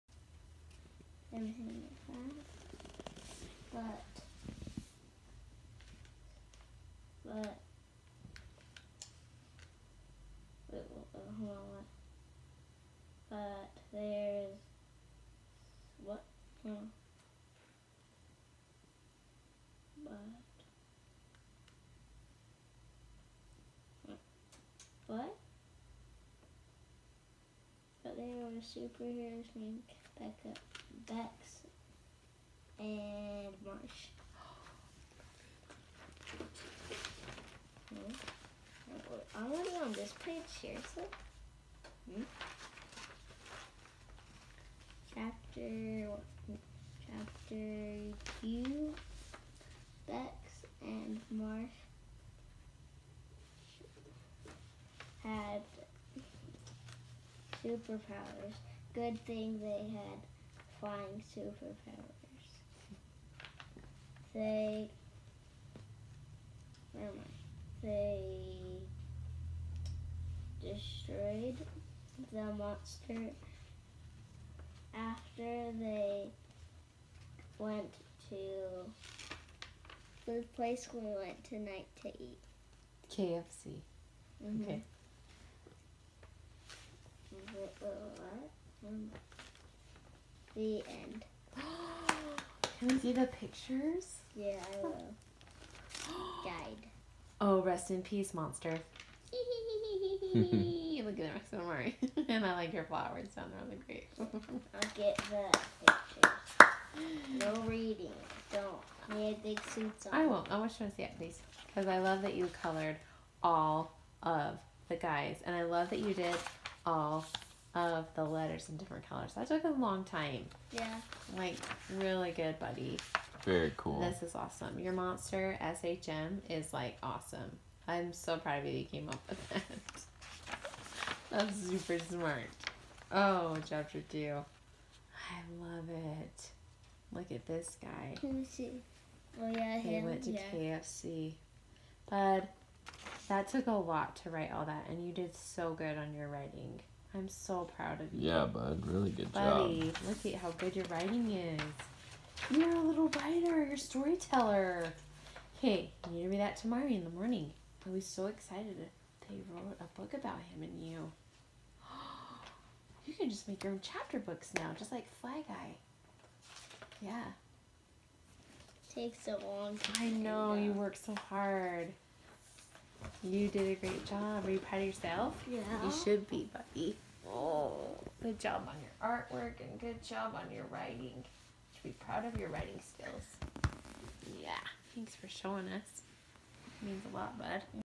Beckahms Story
Beckahm stayed up past bedtime and wrote a super hero story about him and Amari